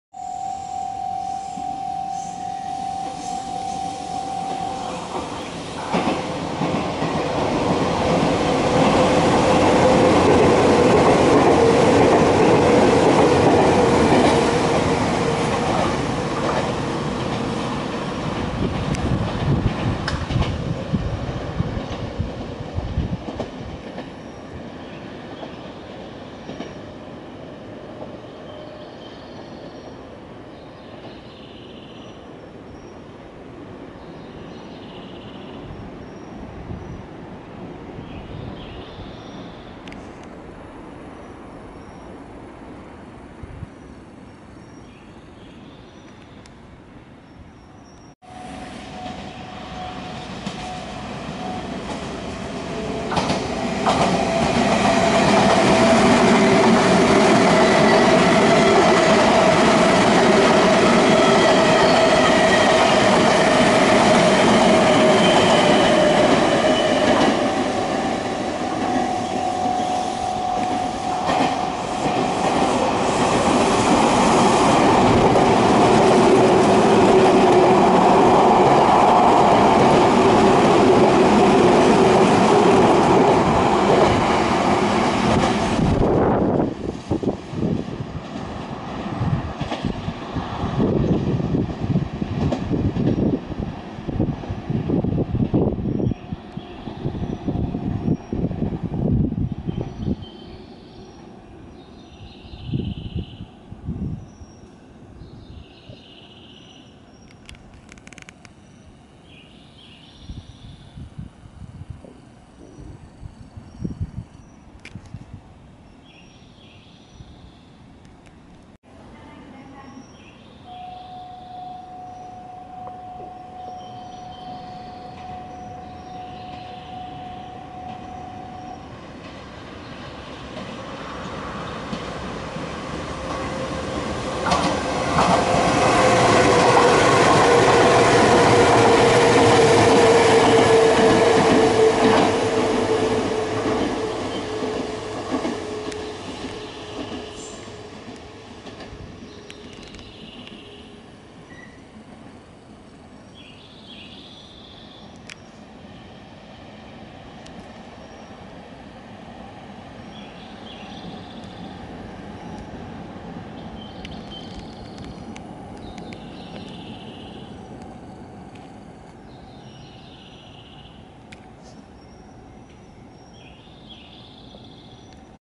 南海線・岸里玉出駅を通過する特急こうや＆泉北ライナーなど
南海線の岸里玉出駅は、岸里駅と玉出駅がくっついて岸里玉出駅になったので、たくさんの路線の電車が往き交います。赤いラインの特急こうやや、泉北ライナーが通過して行きました。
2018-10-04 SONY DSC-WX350